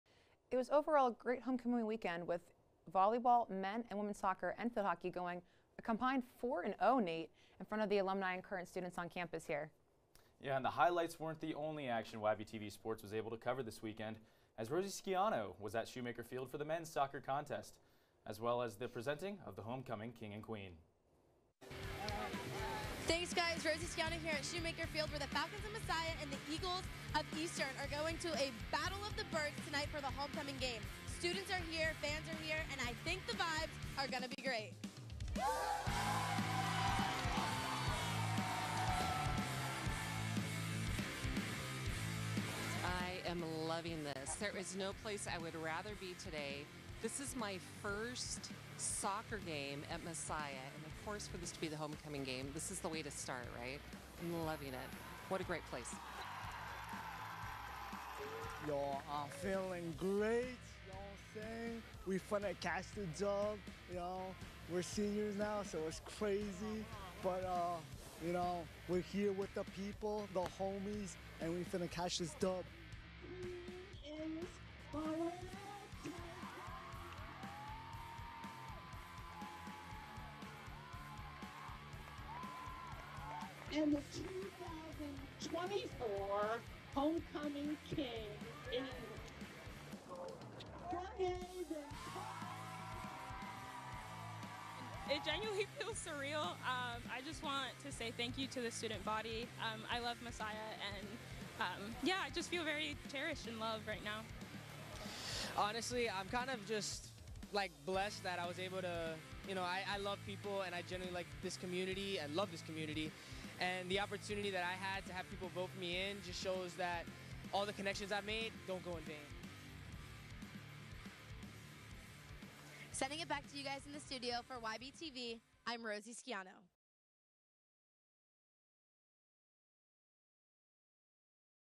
Messiah Mens Soccer Homecoming Game - YBTV Sports
This past weekend, YBTV Sports had the opportunity to cover the Mens Soccer Homecoming game, as well as the crowning of Messiah's Homecoming King and Queen.

Watch our latest full show so you never miss a story, and don't forget to catch us live right here on Youtube, Monday nights at 7!